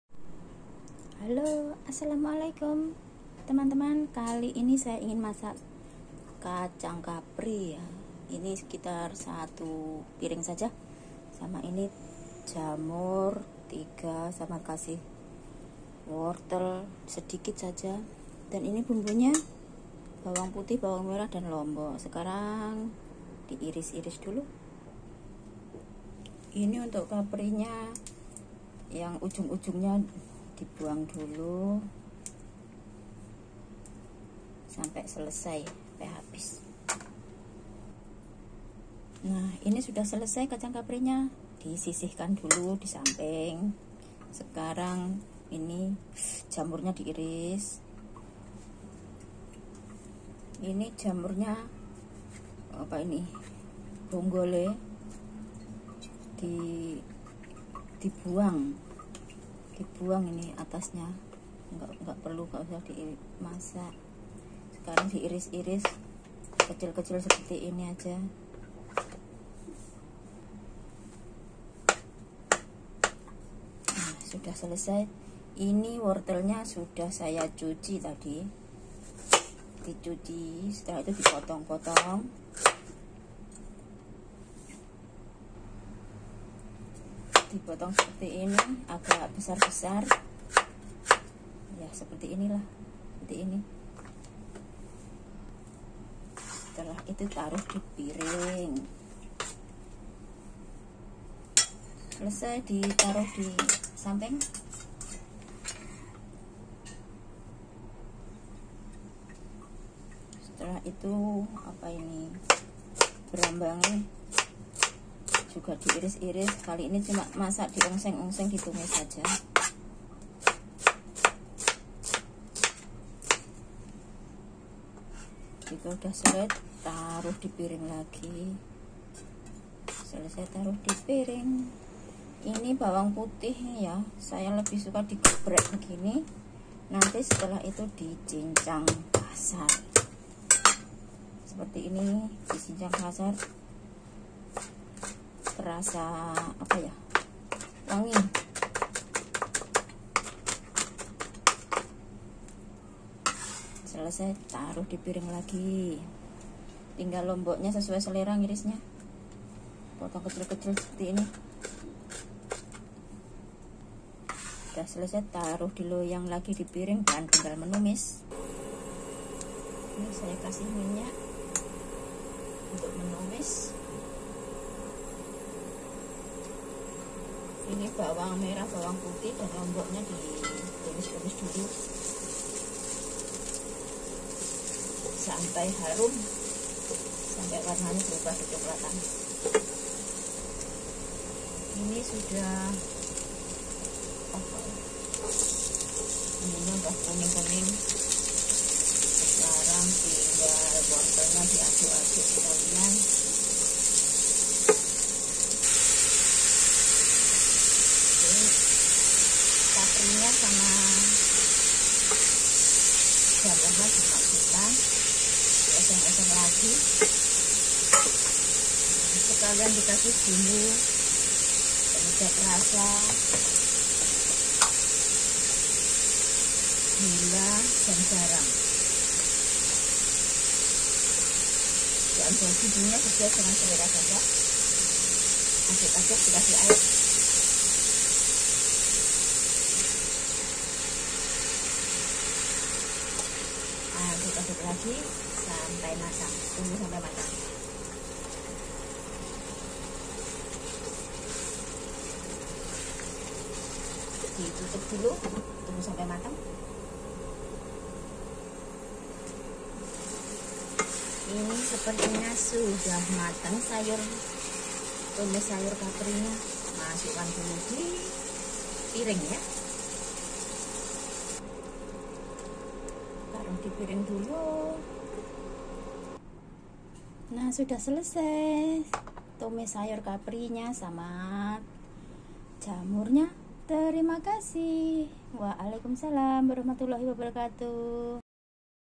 Tumis Kacang Kapri ala TKW Hongkong
Tumis Kacang Kapri
Bahan: kacang kapri, jamur,wortel, bawang merah, bawang putih dan cabe
Semua bahan di bersihkan dan di potong potong, jika sudah selesai siap untuk dimasak, pertama bawang merah, bawang putih dan cabe ditumis sampai harum selanjutnya masukkan wortel tumis lagi, beri sedikit air supaya wortel bisa matang, dan terakhir masukkan kacang kapri dan jamur
Juga beri penyedap rasa, gula dan garam sesuai selera.
Jika sudah matang siap untuk di sajikan.